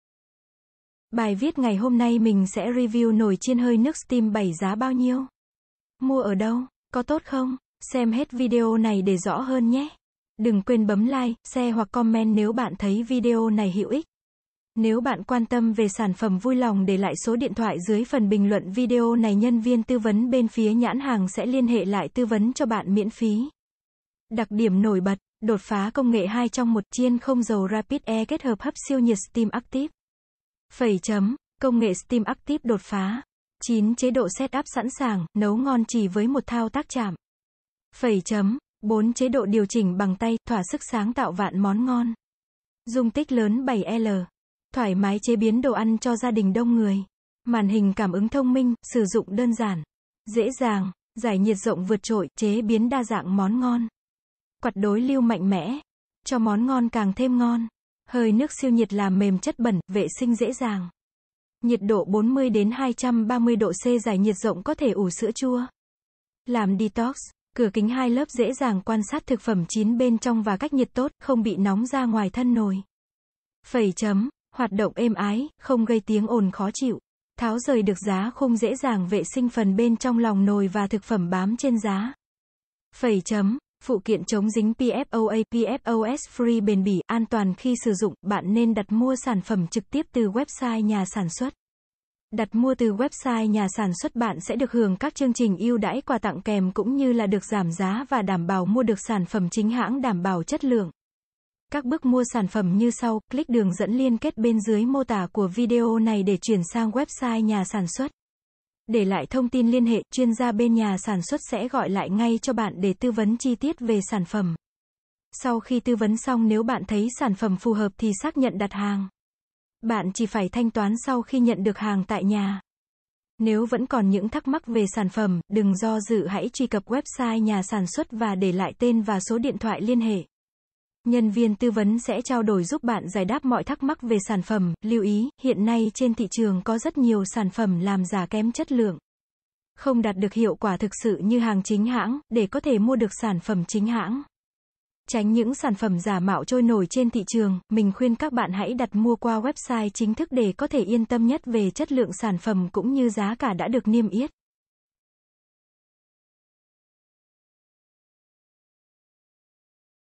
Review Nồi chiên hơi nước STEAM 7 Giá Bao Nhiêu? Mua Ở Đâu? Có tốt không?
Bài viết ngày hôm nay mình sẽ Review Nồi chiên hơi nước STEAM 7 Giá Bao Nhiêu? Mua Ở Đâu? Có tốt không? Nếu bạn quan tâm về sản phẩm vui lòng để lại số điện thoại dưới phần bình luận video này nhân viên tư vấn bên phía nhãn hàng sẽ liên hệ lại tư vấn cho bạn miễn phí
– Đột phá công nghệ 2 trong 1: Chiên không dầu rapid air kết hợp hấp siêu nhiệt steam active.
– Công nghệ Steam Active đột phá.
– **9 chế độ setup sẵn sàng, nấu ngon chỉ với 1 thao tác chạm.
– **4 chế độ điều chỉnh bằng tay, thỏa sức sáng tạo vạn món ngon.
– **Dung tích lớn 7L**, thoải mái chế biến đồ ăn cho gia đình đông người.
– **Màn hình cảm ứng thông minh, sử dụng đơn giản, dễ dàng.
– **Dải nhiệt rộng vượt trội, chế biến đa dạng món ngon.
– **Quạt đối lưu mạnh mẽ**, cho món ngon càng thêm ngon.
– **Hơi nước siêu nhiệt làm mềm chất bẩn, vệ sinh dễ dàng.
– Nhiệt độ 40-230 độ C dải nhiệt rộng có thể ủ sữa chua, làm detox,…
– Cửa kính 2 lớp dễ dàng quan sát thực phẩm chín bên trong và cách nhiệt tốt, không bị nóng ra ngoài thân nồi.
– Hoạt động êm ái, không gây tiếng ồn khó chịu.
– Tháo rời được giá khung dễ dàng vệ sinh phần bên trong lòng nồi và thực phẩm bám trên giá.
– Phụ kiện chống dính PFOA/ PFOS FREE bền bỉ, an toàn khi sử dụng.